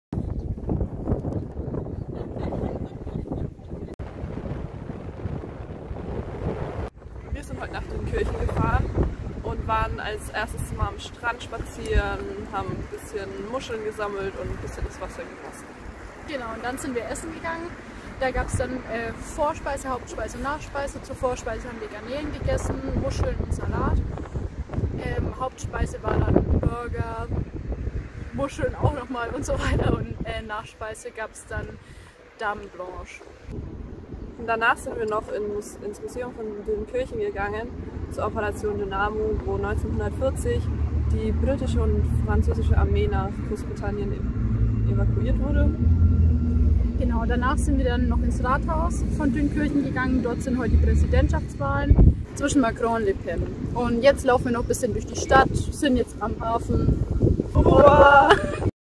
Erasmus+ In Lille tätige Praktikanten besuchen Dunkerque Dünkirchen
Zwei Schülerinnen des BSZ Kulmbach absolvieren zur Zeit ein Praktikum in Lille. Auf dem Kulturprogramm stand heute die Erkundung der nahgelegenen Hafenstadt Dunkerque / Dünkirchen.